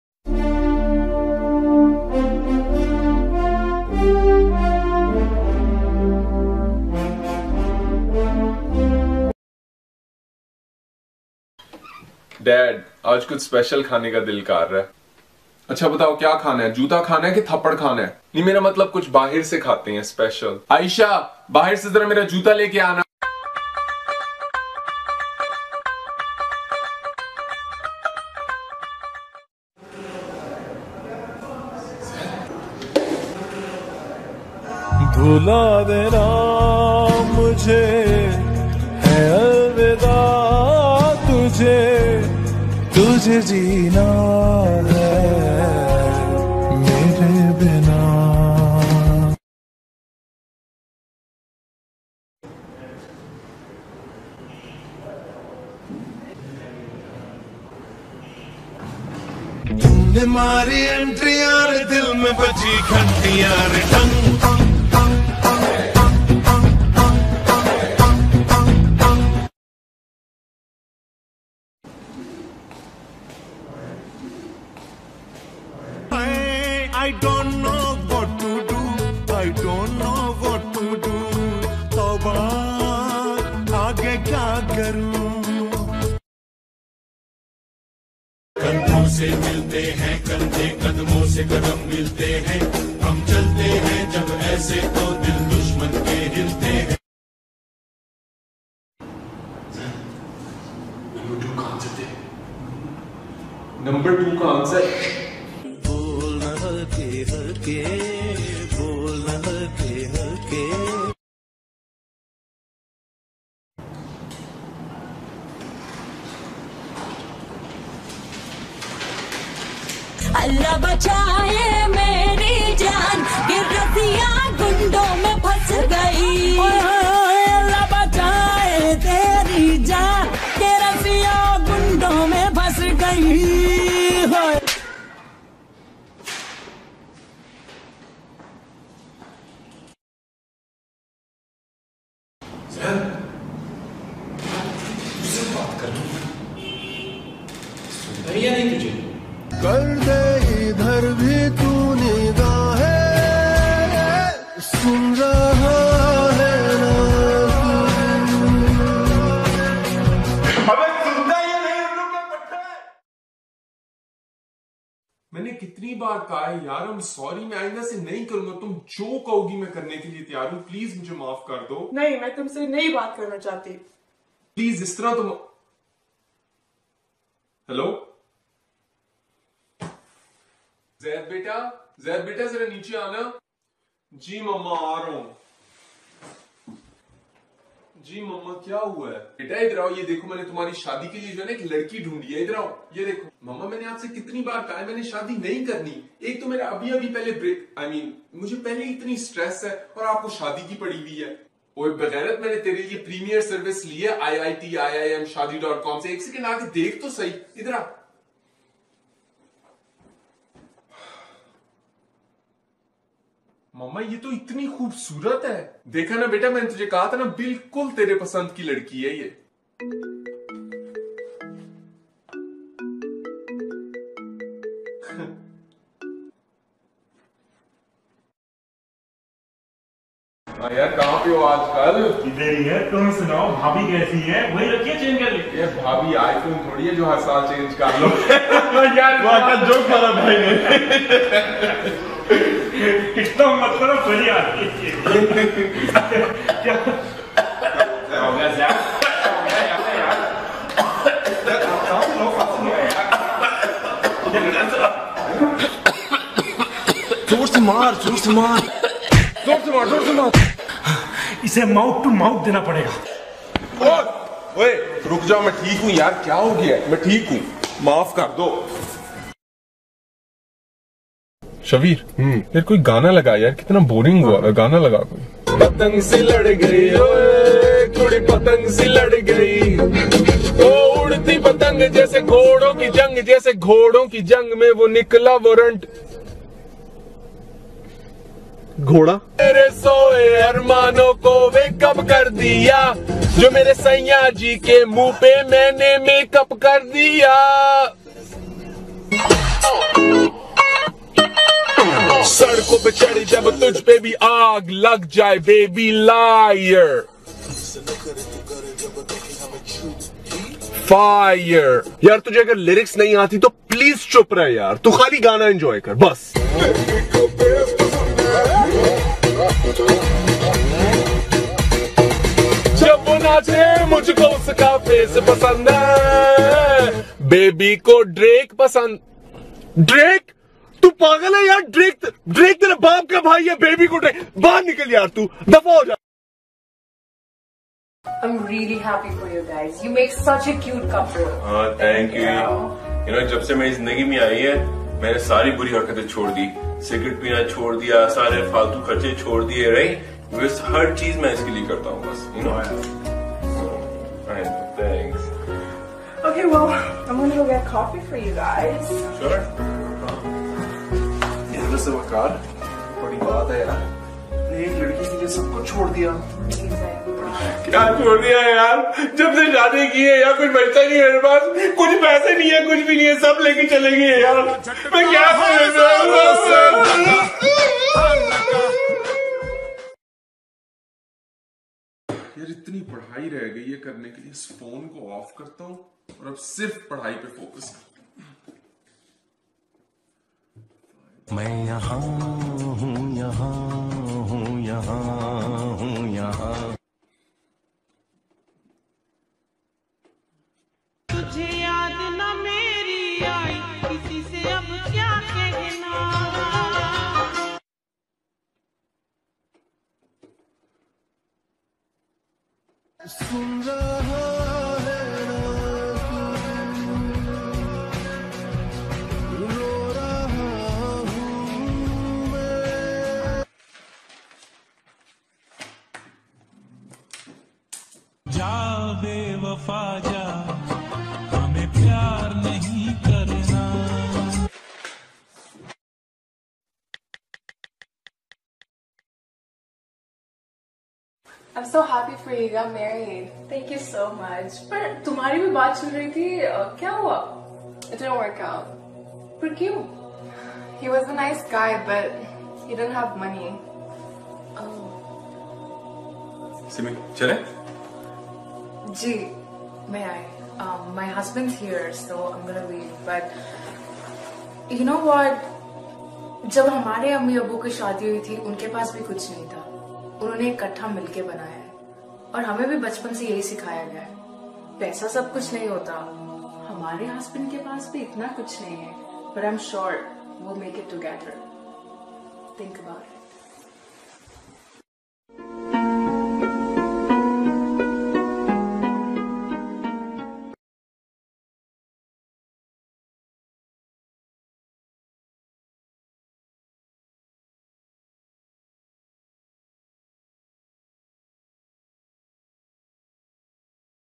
Zaid Ali,Sham Adrees and shahveer jafry Funny Videos collection 2017
This video is a collection of famous YouTuber sham Adrees, shahveer jafry and Zaid Ali